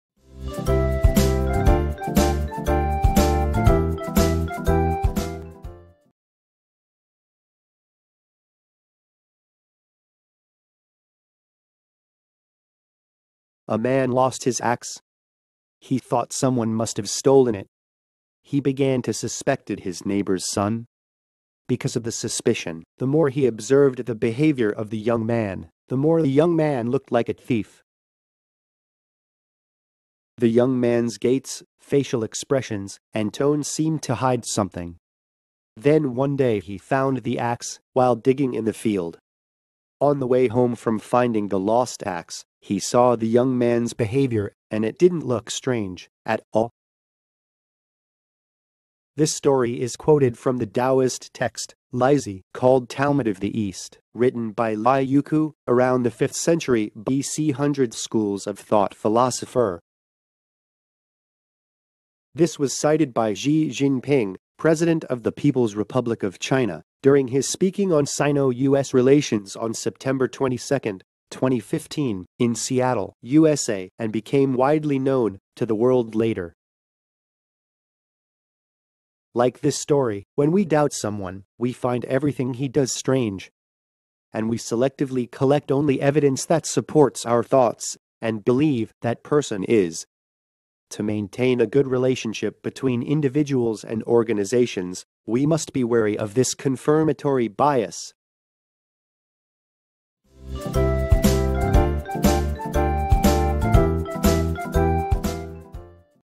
Short Story: A young man suspected of stealing an axe (Liezi)
This story is quoted from the Taoist text [Liezi], called Talmud of the East, written by Lie Yukou, around the 5th century BC Hundred Schools of Thought philosopher. This was cited by Xi Jinping, President of the People's Republic of China, during his speaking on Sino-US relations on September 22, 2015, in Seattle, USA and became widely known to the world later.
Like this story, when we doubt someone, we find everything he does strange. And we selectively collect only evidence that supports our thoughts and believe that person is. To maintain a good relationship between individuals and organizations, we must be wary of this 'confirmatory bias'.